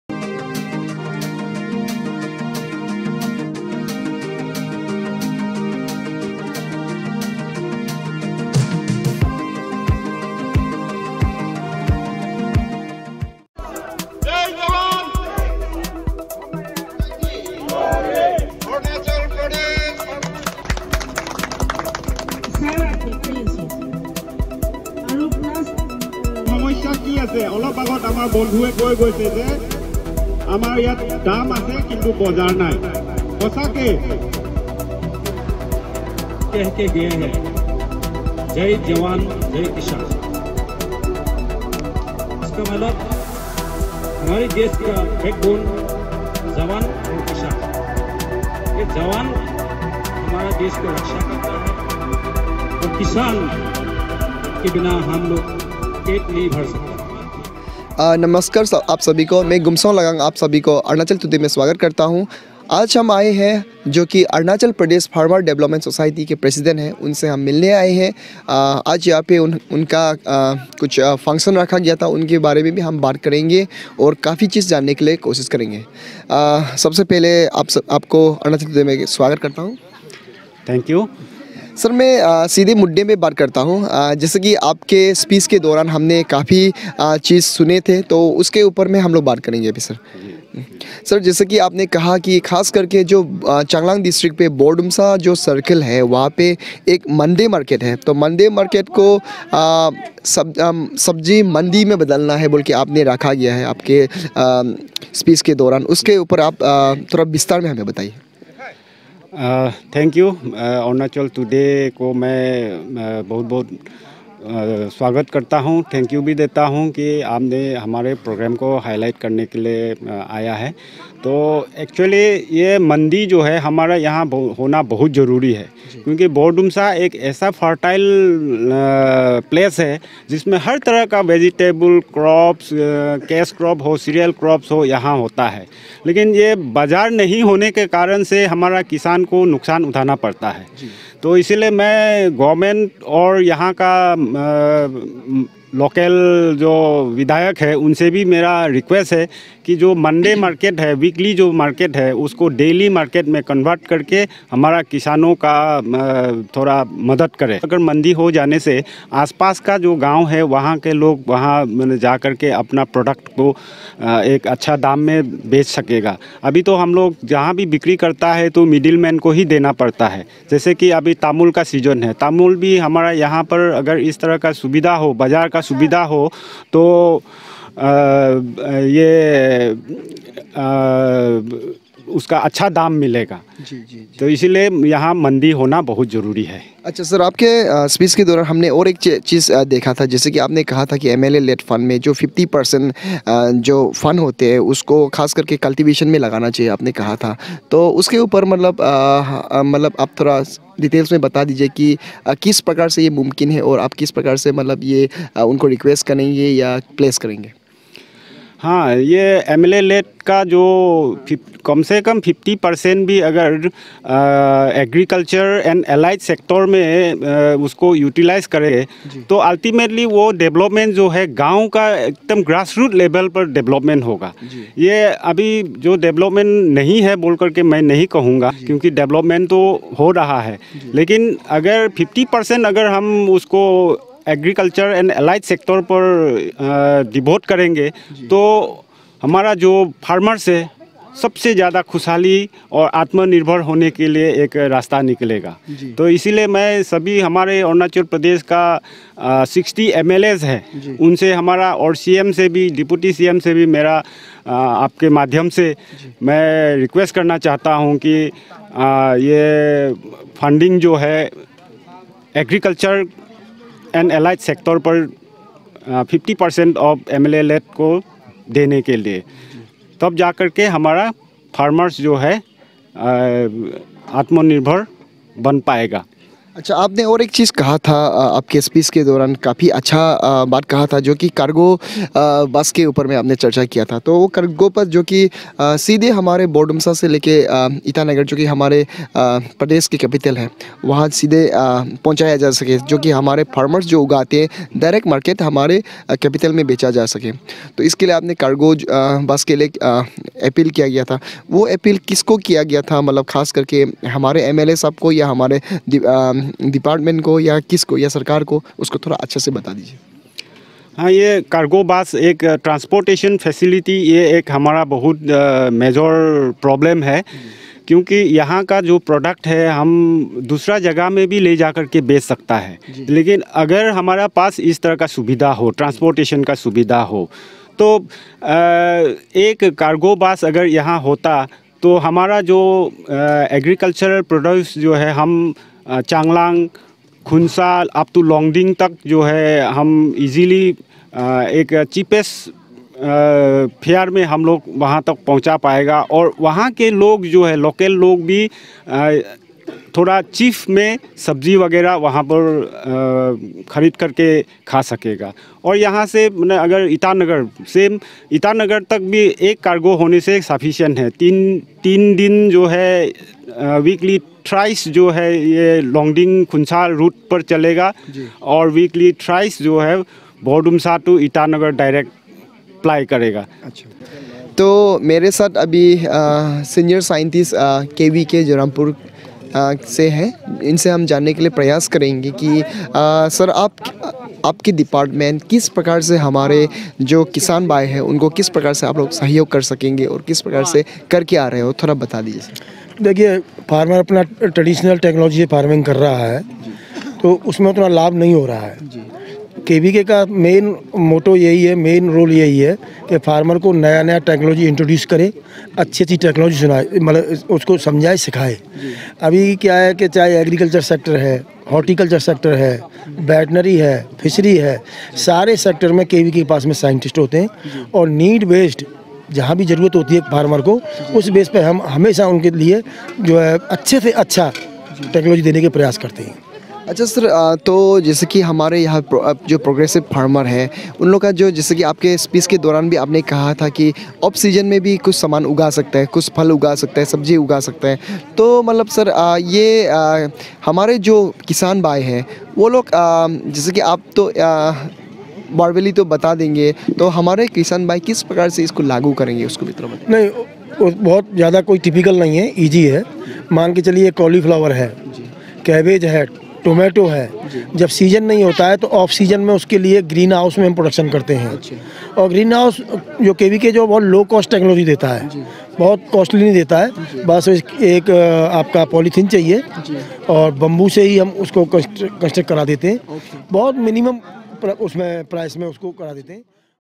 Farmers Development Society urges legislators to use 50% of MLA Lad Fund in Agriculture sector
Arunachal Pradesh Farmers Development Society urges all the legislators to use their 50% of MLA Lad Fund in Agriculture sector
Arunachal Pradesh Farmers Development Society observed National Farmers Day (Kisan Diwas) at Bordumsa